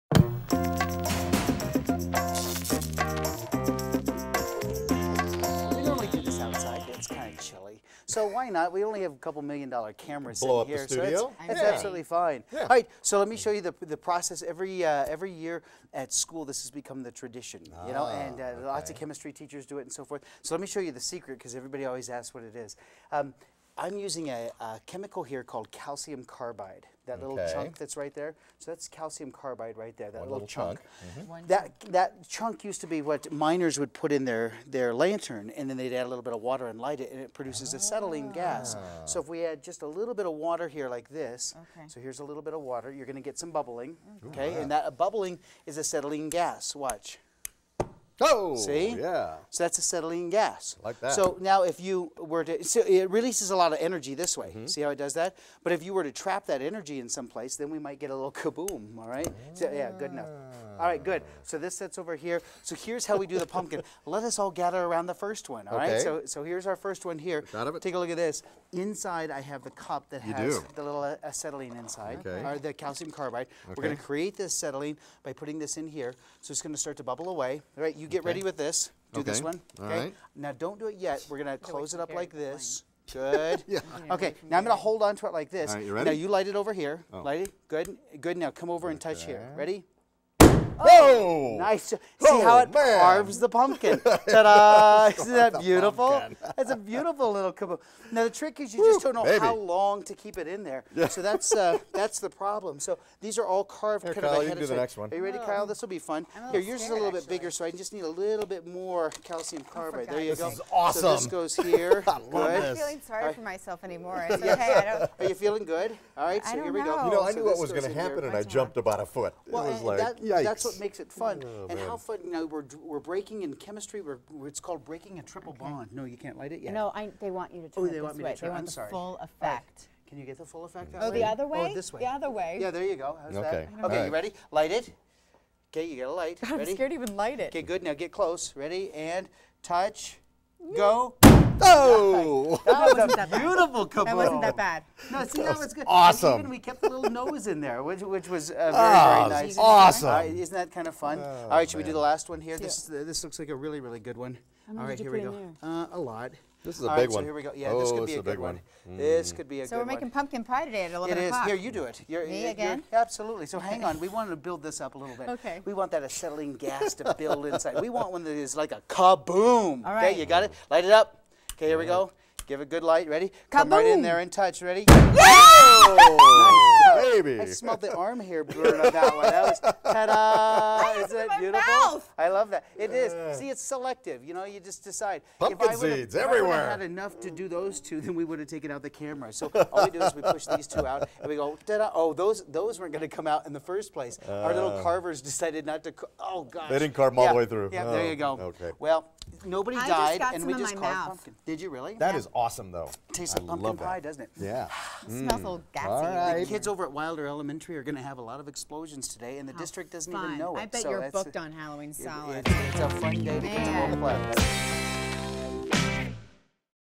Exploding Pumpkins - Cool Science Experiment
Just consider it a new way to carve a pumpkin at Halloween... the pumpkin is actually carved in the traditional way, but the pieces of the jack-o-lantern face get pushed back into place. A chemical reaction is triggered inside the pumpkin and the previously carved pieces are literally blown out of the pumpkin. If you didn't know that the pumpkin had been carved already, it would appear as if you magically carved the pumpkin with a single eruption. What about the science part of this? This method is a favorite among chemistry teachers, who use the demo to illustrate the energy released when you break a triple bond in a molecule of acetylene. About Steve Spangler Science...